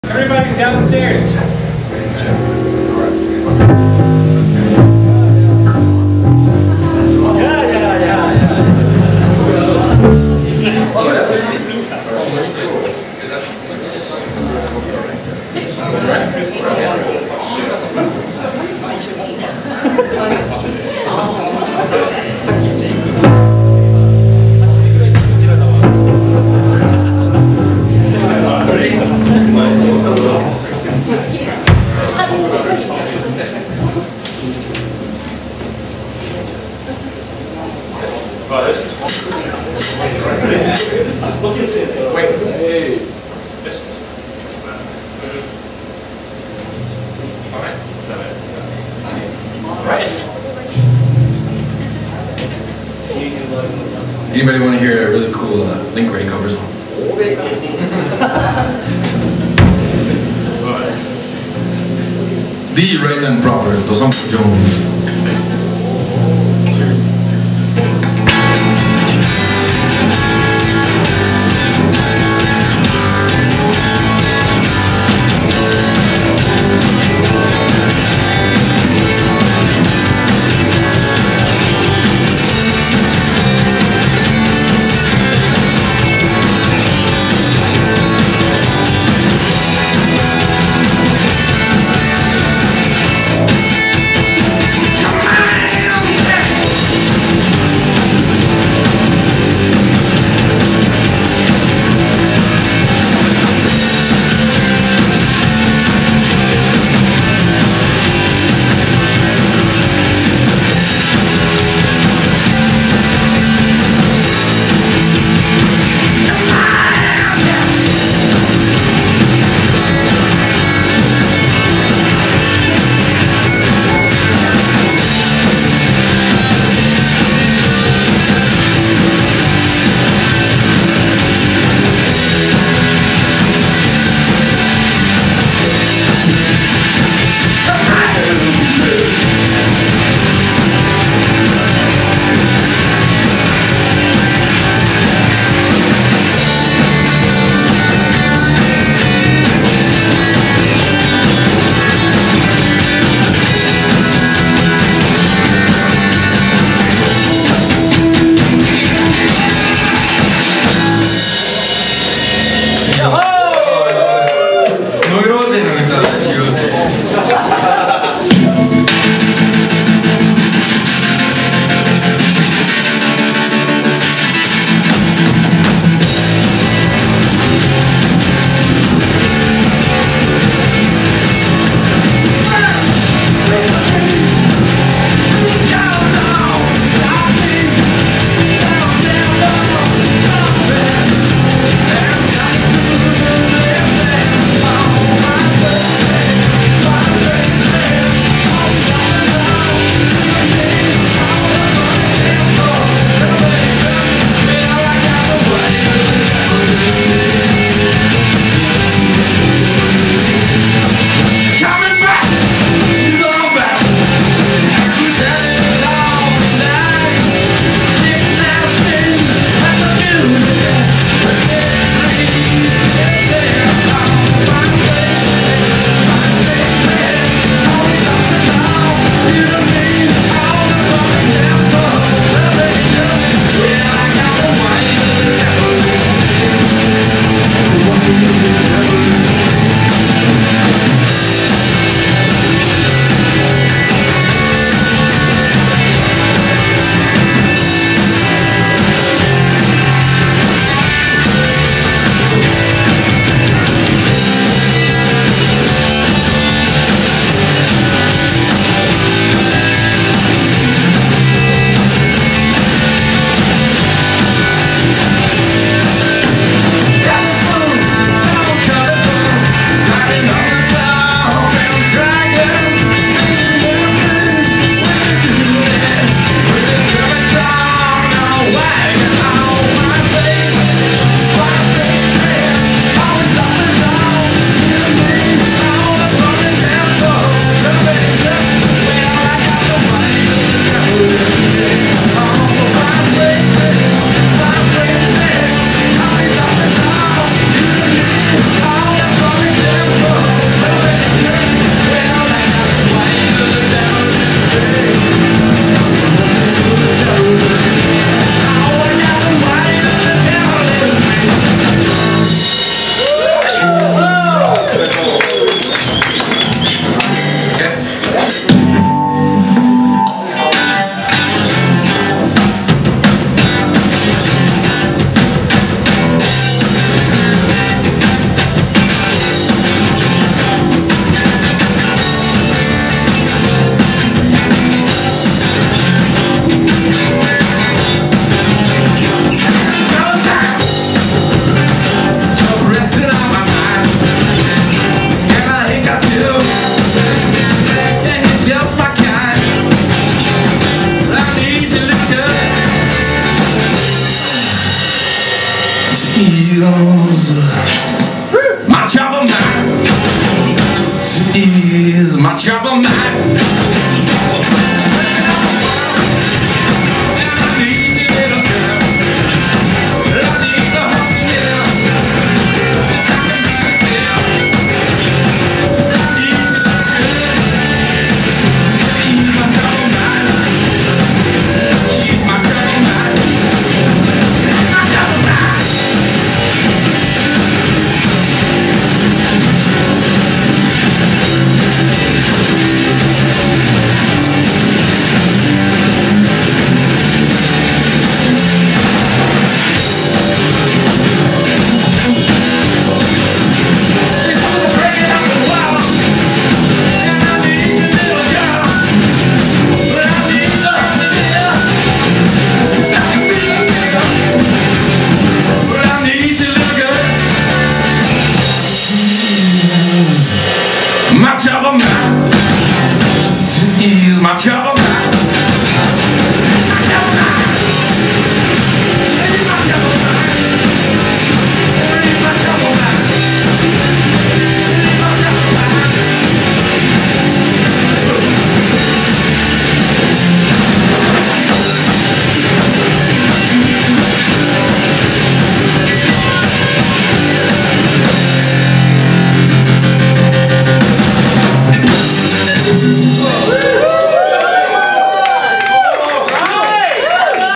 Thee Right and Proper Dosanko Jones 2007 1
Dosanko Jones at the "Dead Rock Stars II" festival part 1.  Garage punk medley.